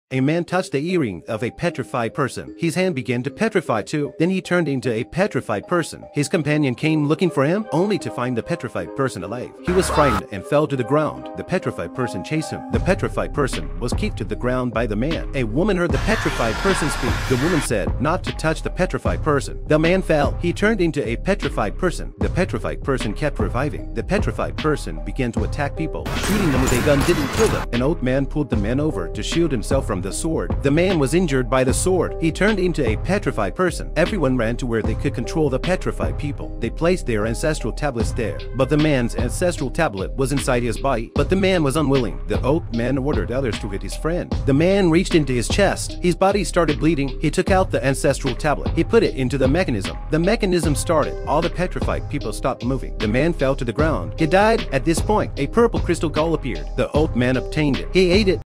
The earring turns people into petrified beings.
The man just touched the earring of the petrified person, and he turned into a petrified being. All the petrified beings start to awaken and attack humans. How can humans break the deadlock?